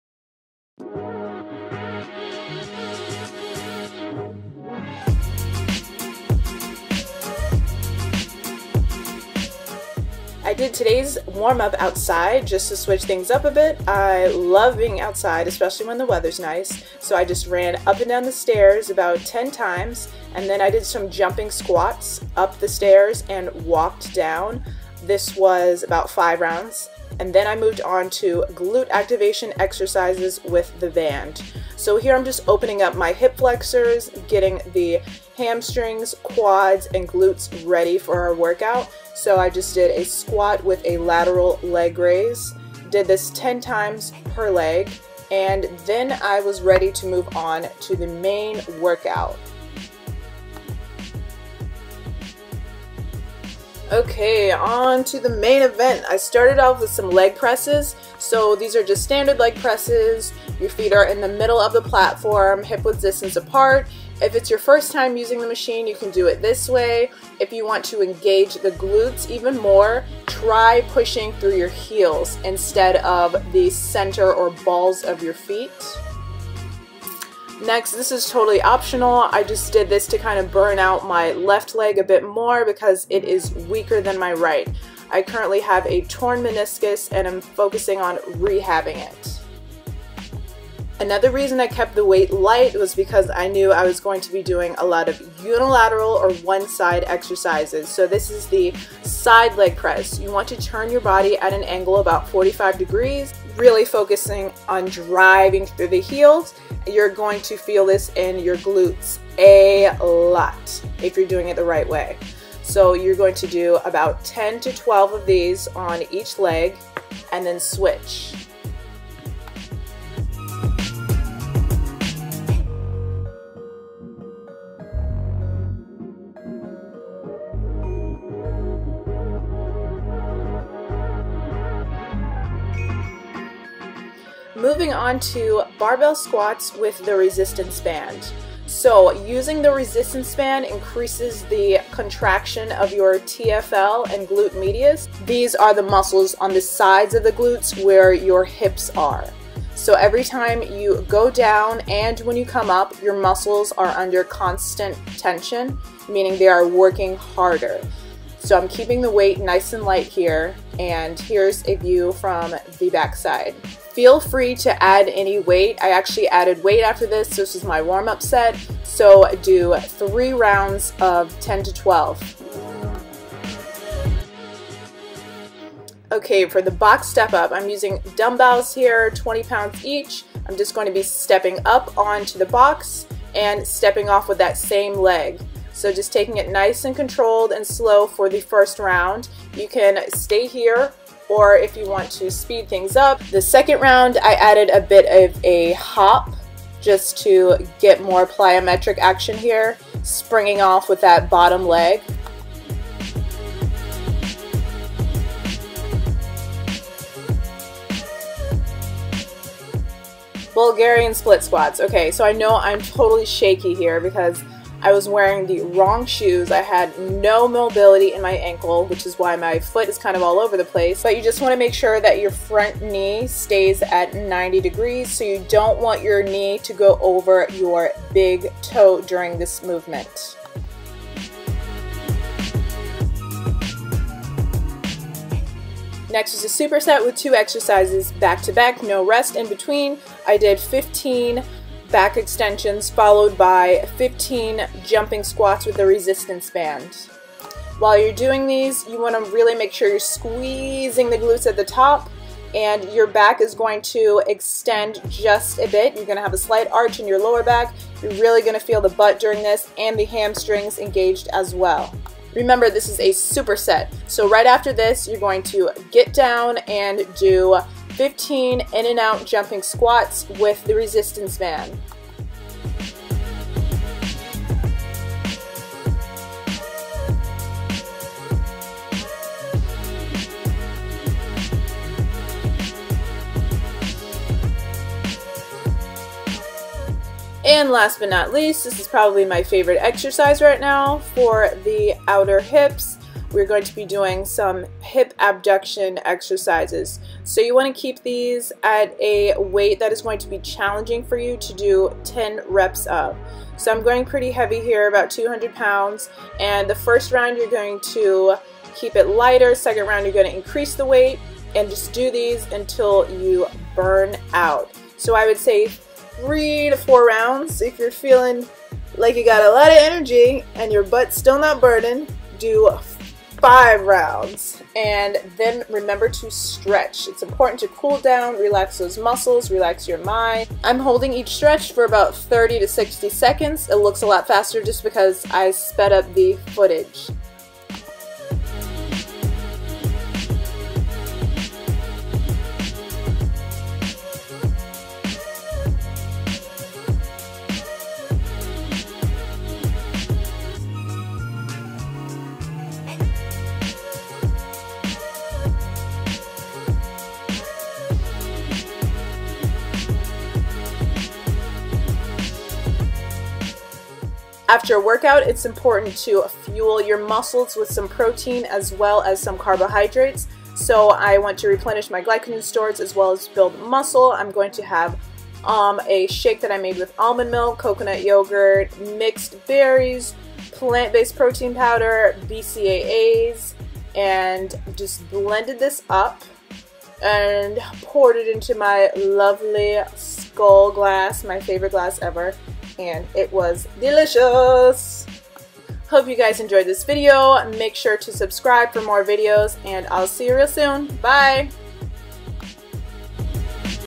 NOT YOUR AVERAGE BOOTY WORKOUT | Training GLUTES
Hello! This is a full leg and glute workout; hope you enjoy!